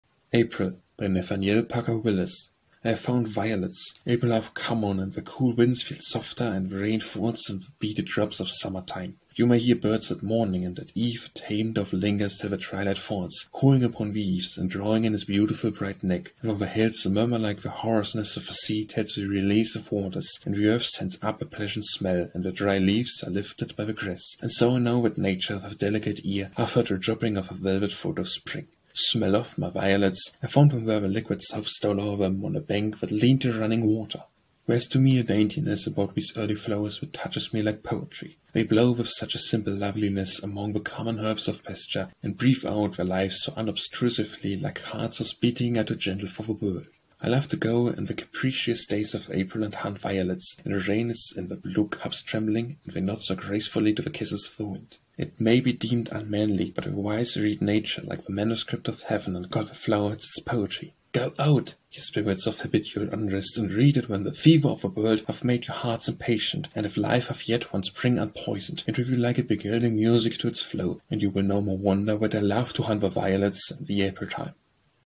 April - Nathaniel Parker Willis
April
Nathaniel Parker Willis

    I have found violets. April hath come on,
    And the cool winds feel softer, and the rain
    Falls in the beaded drops of summer time.
    You may hear birds at morning, and at eve
    The tame dove lingers till the twilight falls,
    Cooing upon the eaves, and drawing in
    His beautiful bright neck, and from the hills,
    A murmur like the hoarseness of the sea
    Tells the release of waters, and the earth
    Sends up a pleasant smell, and the dry leaves
    Are lifted by the grass - and so I know
    That Nature, with her delicate ear, hath heard
    The dropping of the velvet foot of Spring.
    Smell of my violets! I found them where
    The liquid South stole o'er them, on a bank
    That lean'd to running water. There's to me
    A daintiness about these early flowers
    That touches me like poetry. They blow
    With such a simple loveliness among
    The common herbs of pasture, and breathe out
    Their lives so unobtrusively, like hearts
    Whose beatings are too gentle for the world.
    I love to go in the capricious days
    Of April and hunt violets; when the rain
    Is in the blue cups trembling, and they nod
    So gracefully to the kisses of the wind.
    It may be deem'd unmanly, but the wise
    Read nature like the manuscript of heaven
    And call the flowers its poetry. Go out!
    Ye spirits of habitual unrest,
    And read it when the "fever of the world"
    Hath made your hearts impatient, and, if life
    Hath yet one spring unpoison'd, it will be
    Like a beguiling music to its flow,
    And you will no more wonder that I love
    To hunt for violets in the April time.